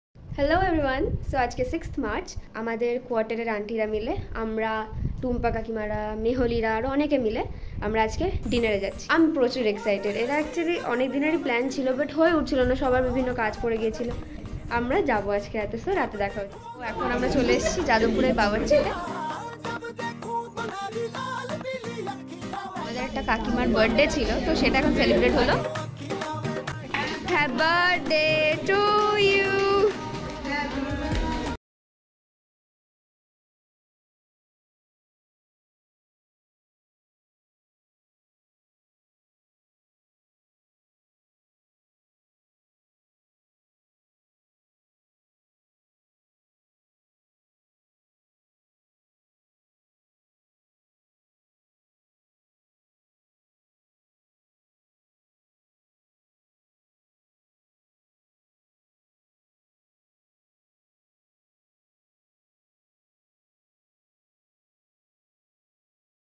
My first content over YouTube | A day spent with the closed ones | _Foody_wood_
I'm Sohini Pal, from Kolkata, West Bengal. I'm here on YouTube to share all the precious and remarkable memories, I'm on my way to create, in the future. Hope to have a wonderful journey with you all✨